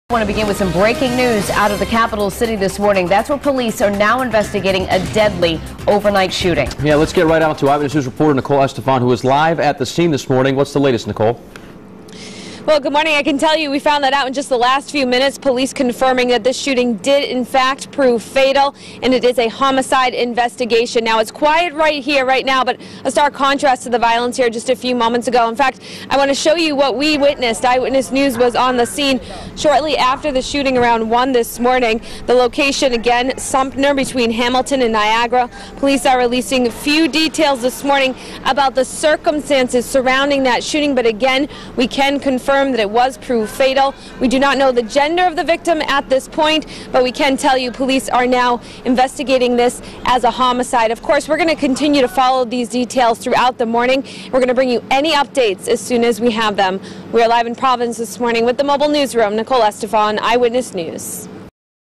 Teen killed in overnight shooting
Providence Police are searching for a suspect in a late night homicide.